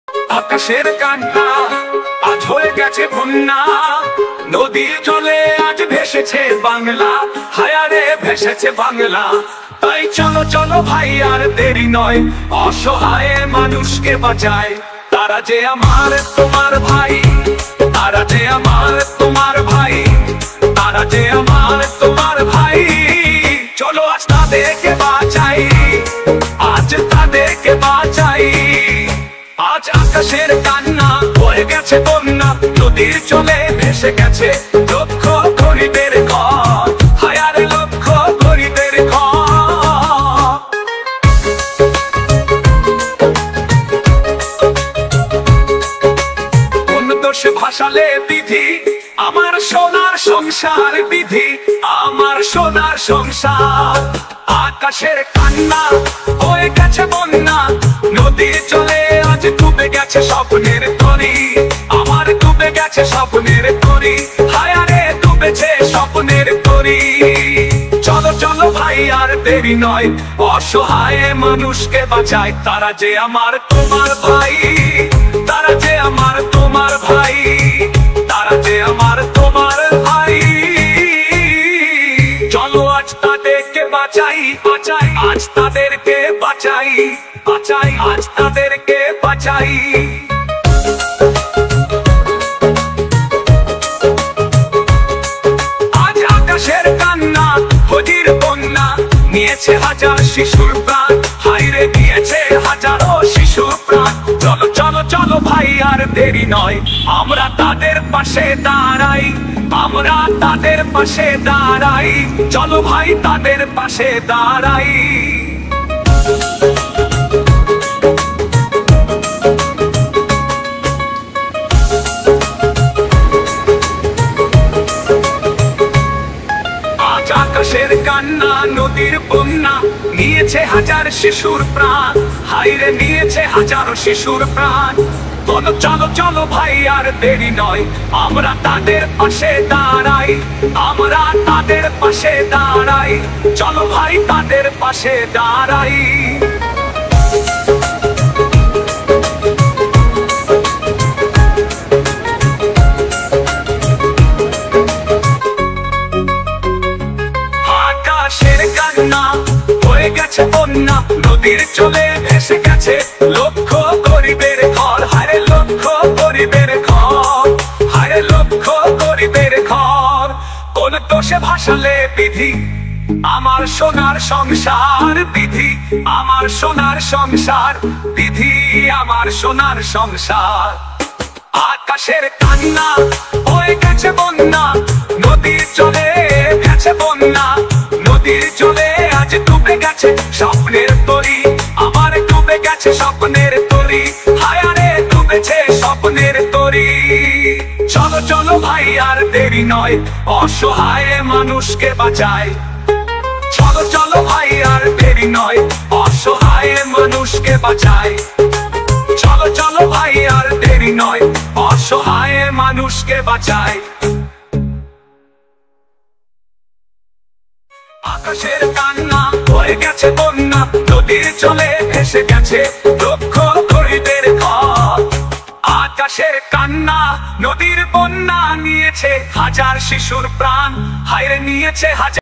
⛈️😭😭😭বন্যা কবলিত আজকের পাঁশকুড়া ঘাটাল এ কেমন অবস্থা 😭😭সরাসরি দেখুন
বন্যা কবলিত পাশকুড়া ঘাটাল
 আজকে কেমন আছে পাঁশকুড়া ঘাটাল